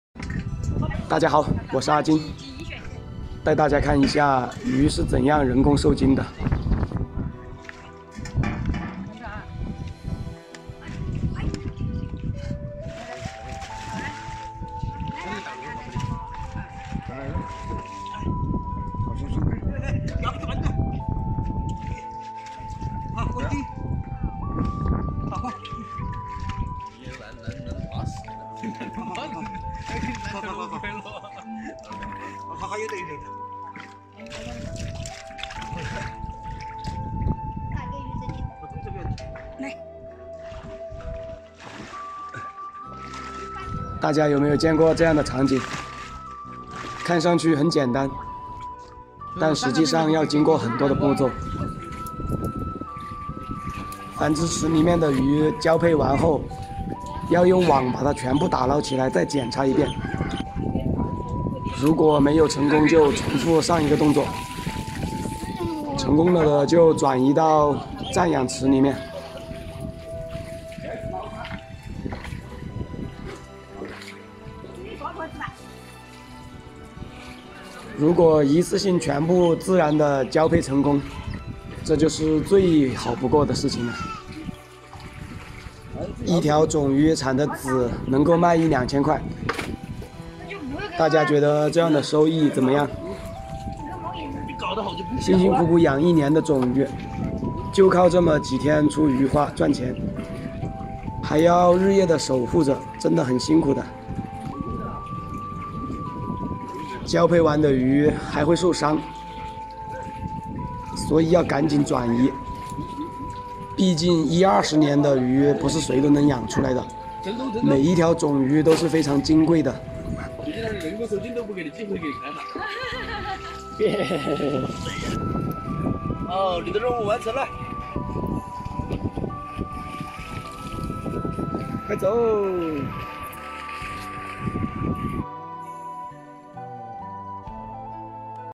价值2000块的大草鱼，人工授精场面紧张，一大盆受精卵
洞庭湖区的种鱼养殖户，大草鱼养了一二十年，一条大的雌鱼价值2000块。繁殖失败后用人工授精，一人抱一只，力气活，授精就在一瞬间。一大盆受精卵，孵化出来就是钱啊！ 繁殖完成后的种鱼，要及时让它们休养，毕竟这么贵的种鱼很难养出来，要格外用心养殖。